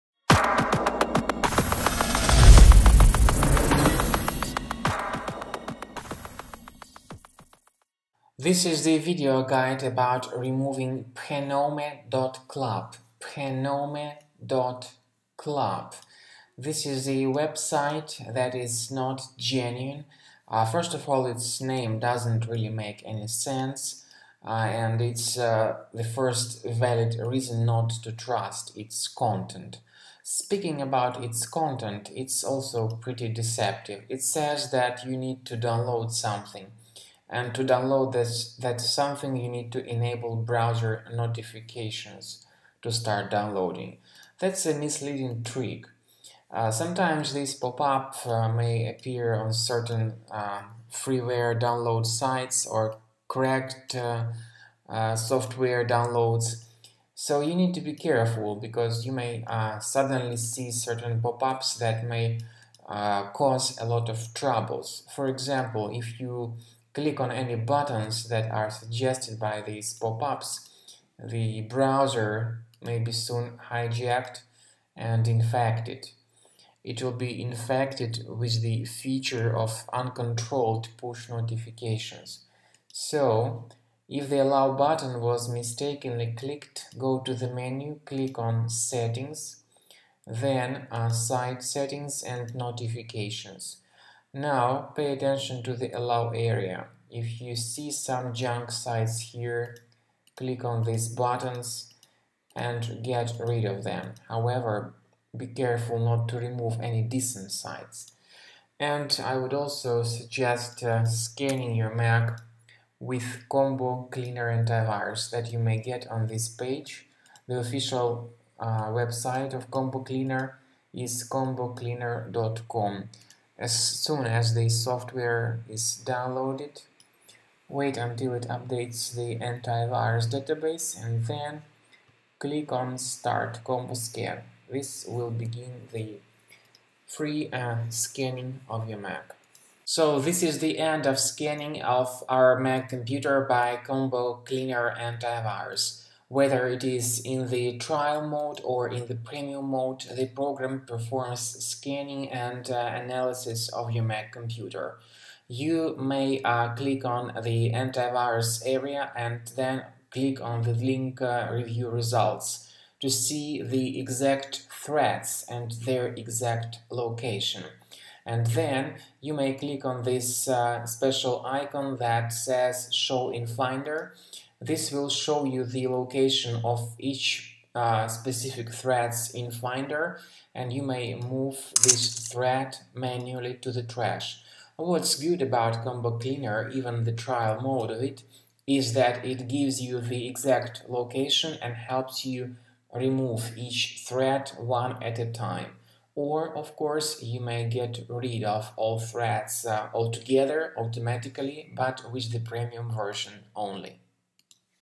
Phenome.club pop-ups - how to delete?
Phenome.club is a junk page displaying a wide range of uncontrolled web push notifications. Delete them with Combo Cleaner Antivirus or follow the manual suggestions in the video.